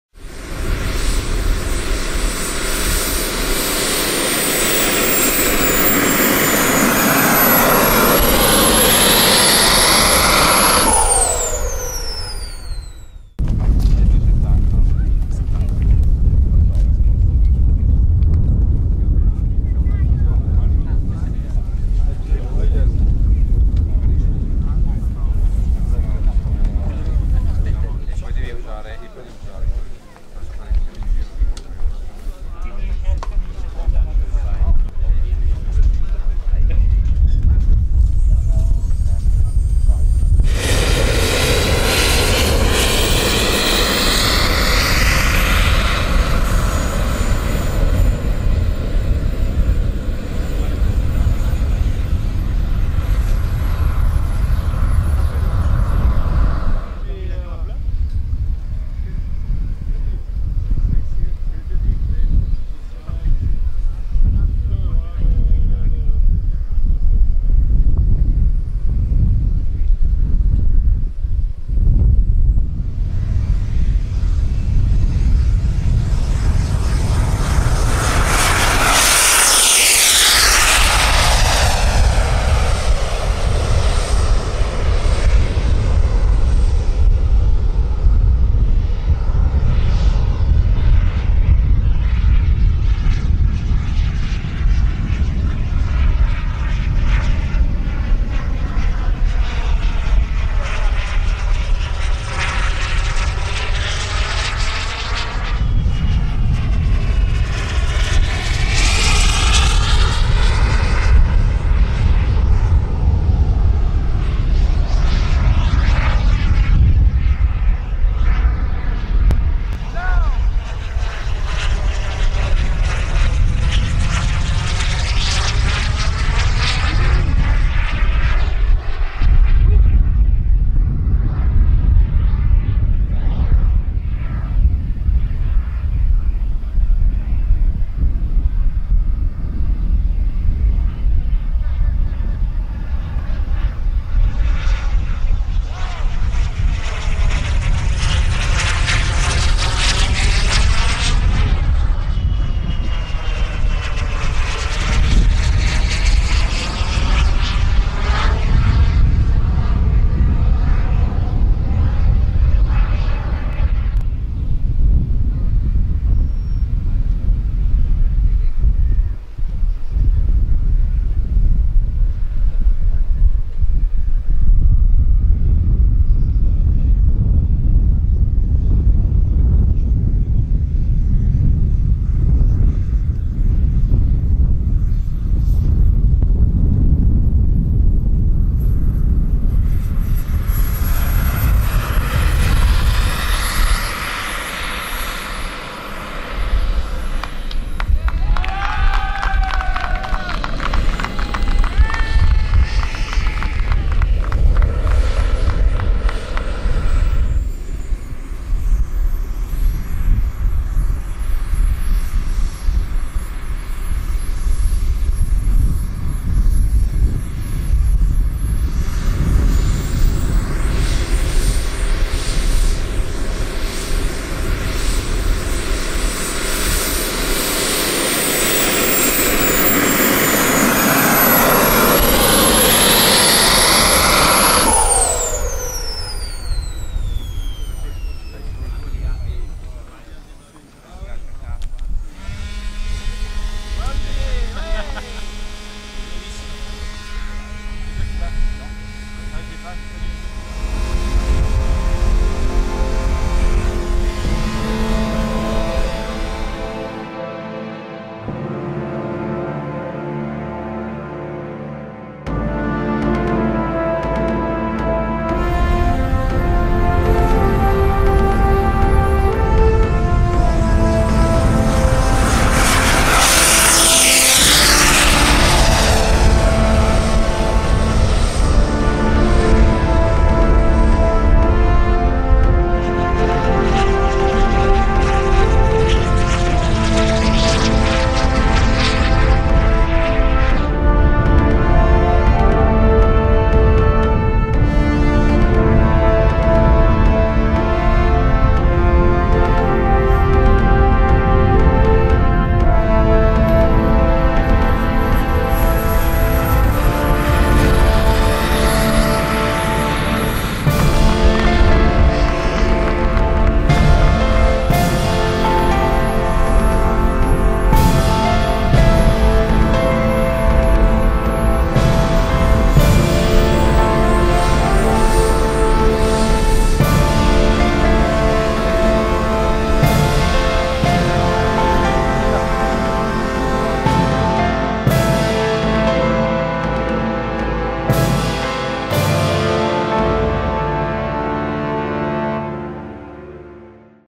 Fouga CM. 170 Magister Oliver Maugeard Team France JWM ITALY 2023
Fouga CM.170 Magister, Oliver Maugeard Jet Team France
5th place in category 15 kg